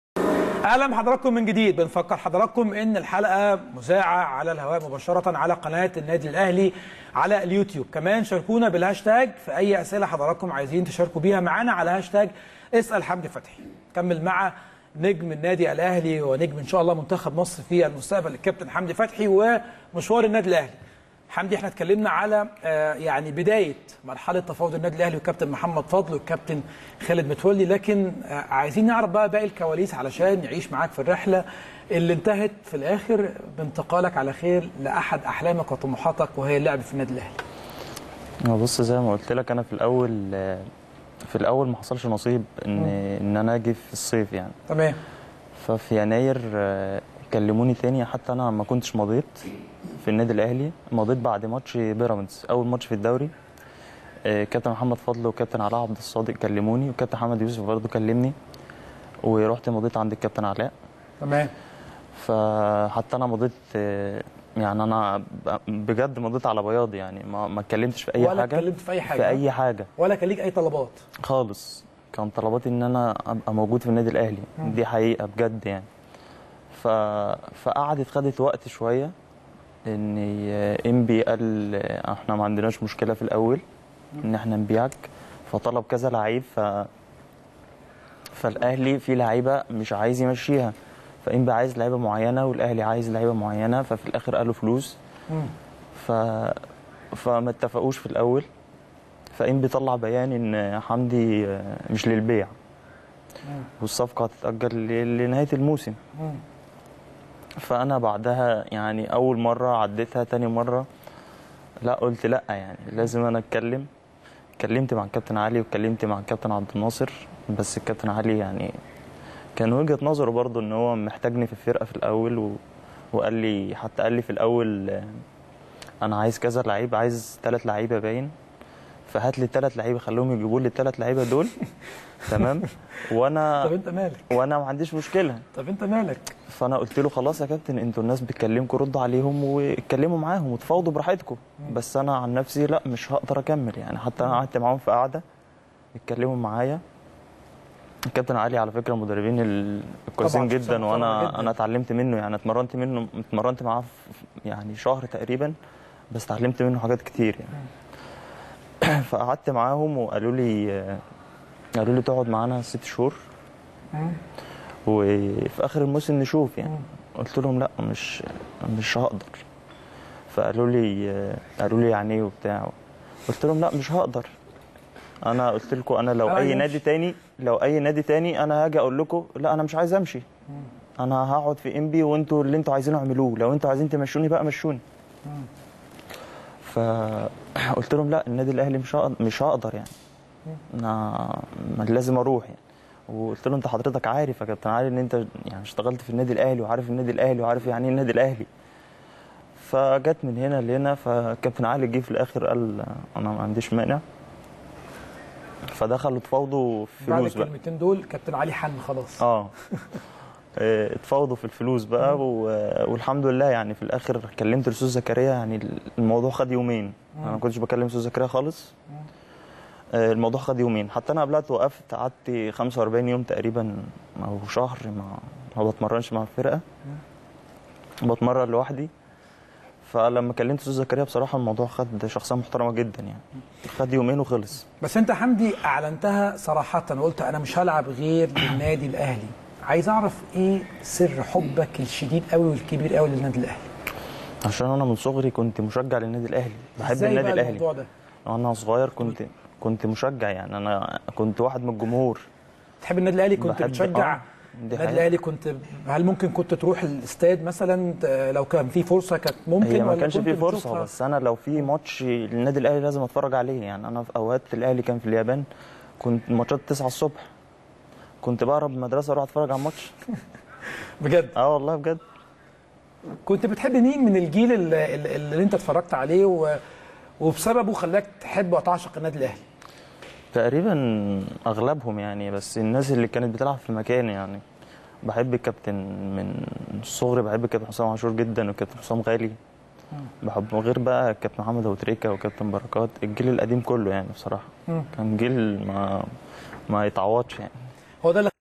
حمدي فتحي: فتحي وقعت للنادي الأهلي على بياض ولم يكن لي اي طلبات إلا حاجه واحدة؟
التردد: 12341 أفقي.
القمر: نايل سات.
معامل الخطأ: 5/6.
معدل الترميز: 27500